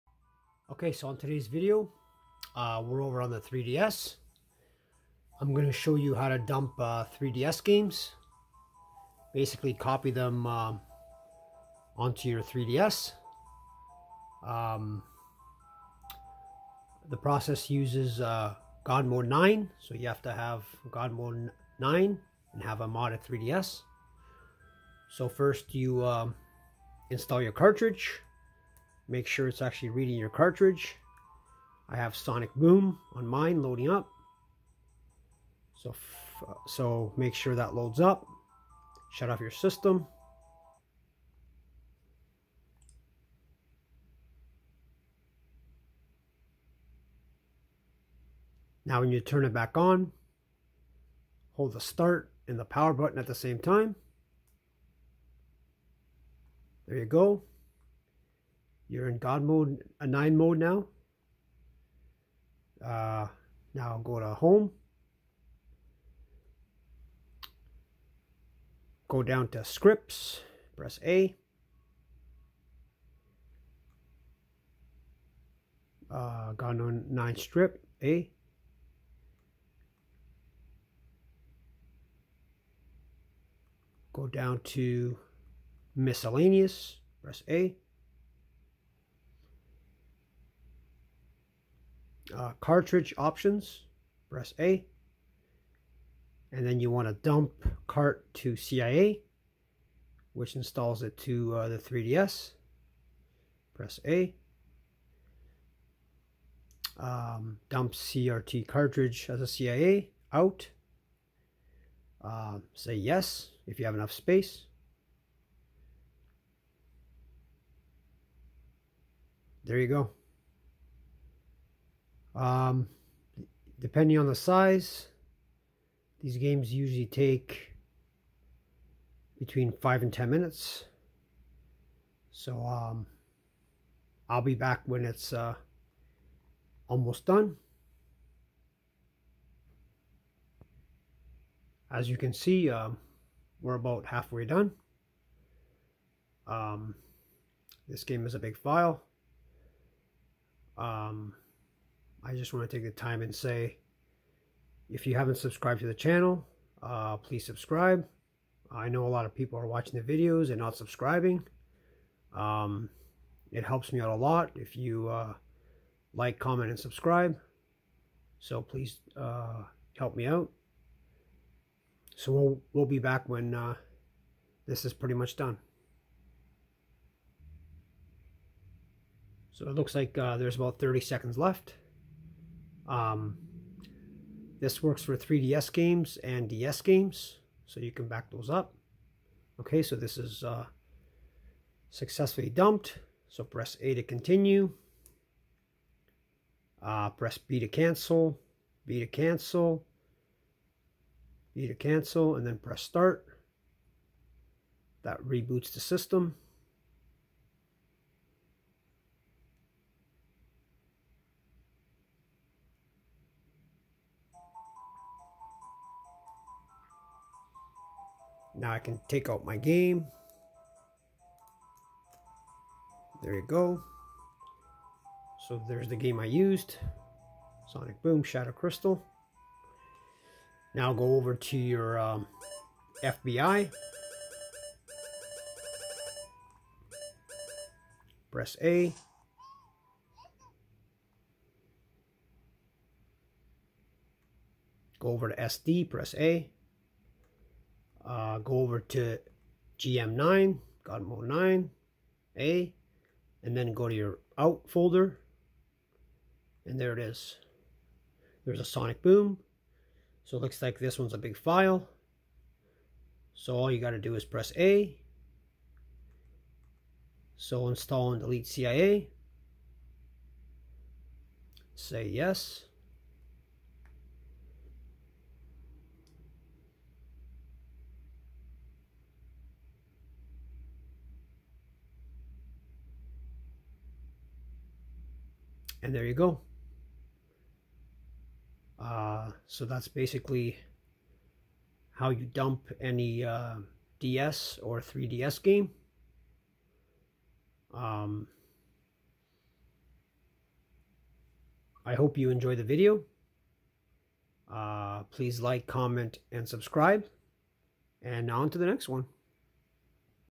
How to Dump / Copy DS & 3DS Games to a Modded Nintendo 3DS!
Dump DS & 3DS Games to a Modded Nintendo 3DS!